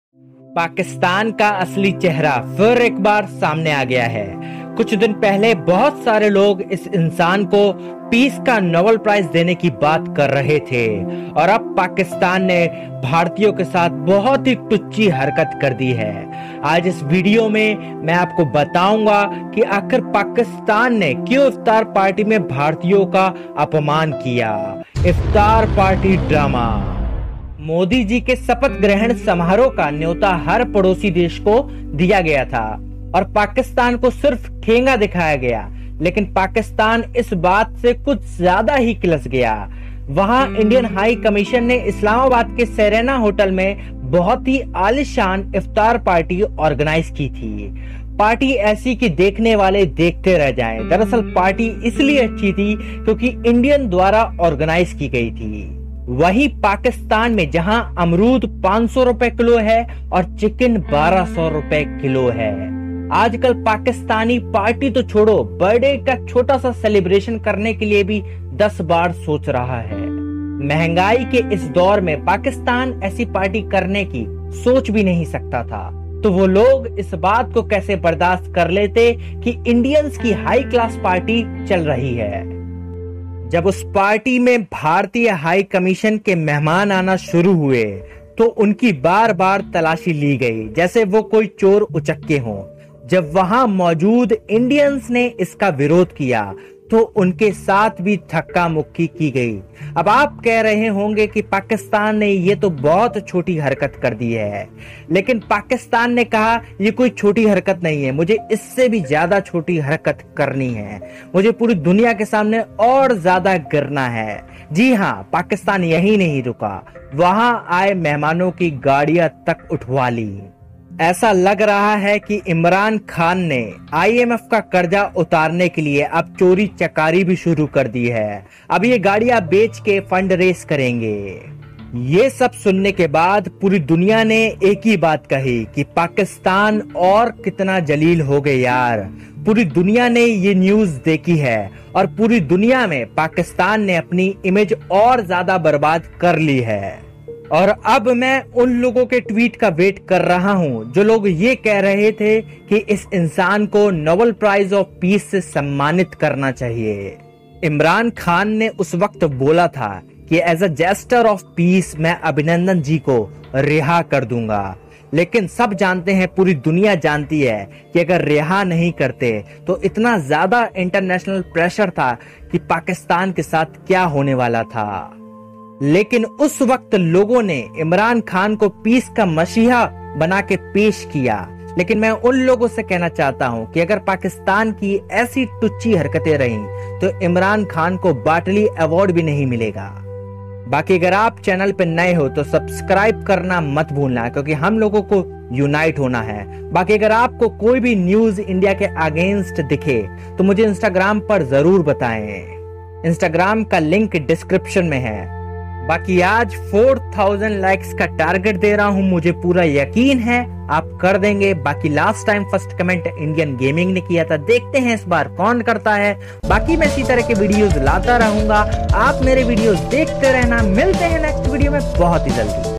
Pakistan insulted indian in iftar party | अब मोदी के खौफ में है पूरा पाकिस्तान देखलो
Pakistan iftar party drama | तो इसलिए भारतीयों का अपमान किया पाकिस्तान ने

Pakistan made fun of iftar party organised by indian high commission so in this video i explained whole scenario

SUPPORT TRUE ARMY WE WILL SHOUTOUT YOUR NAME (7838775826) PAYTM NUMBER

FACEBOOK GROUP TRUE ARMY

INSTAGRAM(JOIN TRUE ARMY)